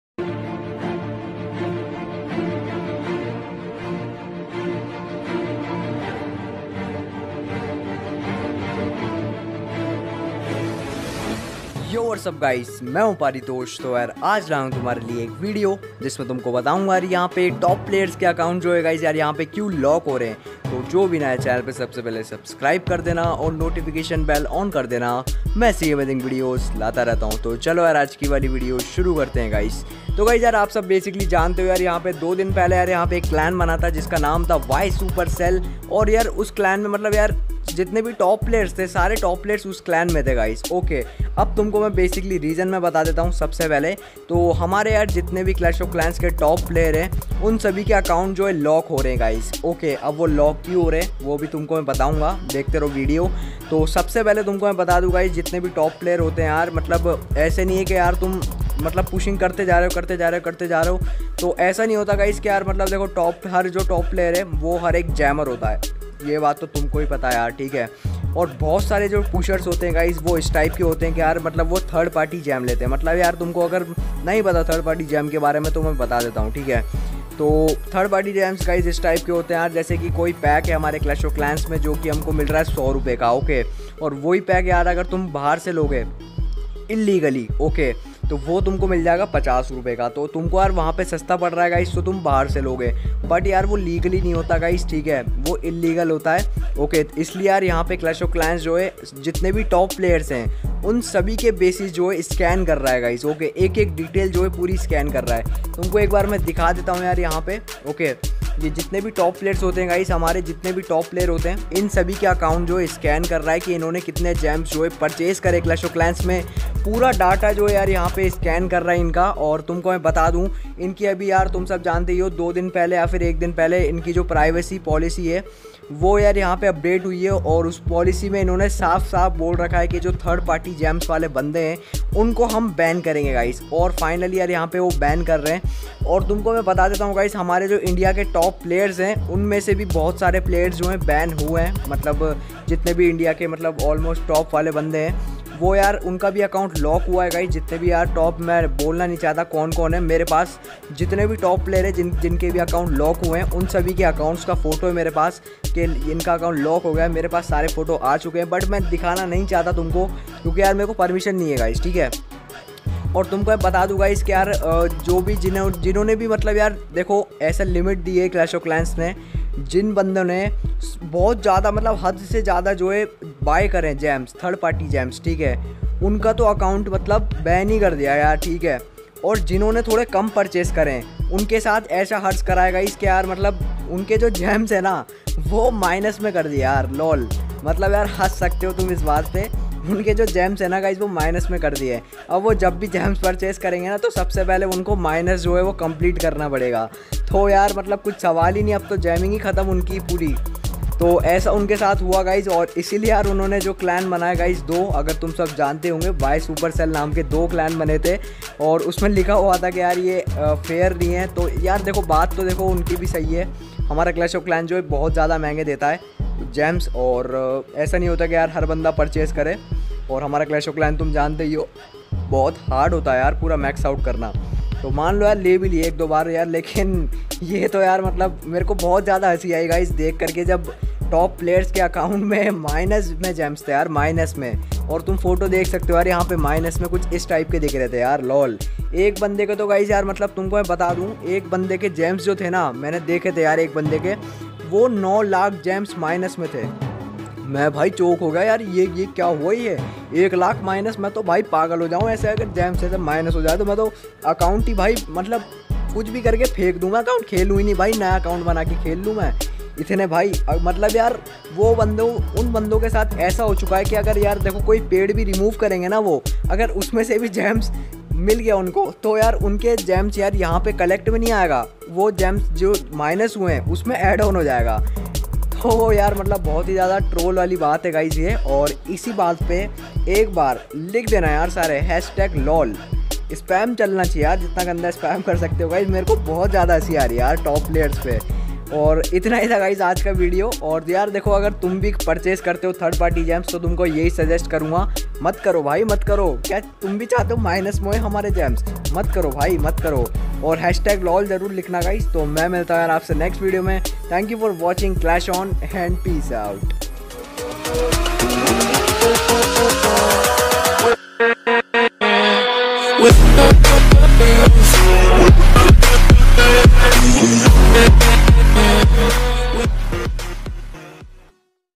WHY SUPERCELL LOCK TOP PLAYERS IN CLASH OF CLANS
Ji guys to ajj me apko bata ne wala hu coc wale top player ke id lock kyu kr rahe he to video puri dak na and like share subscribe kr na.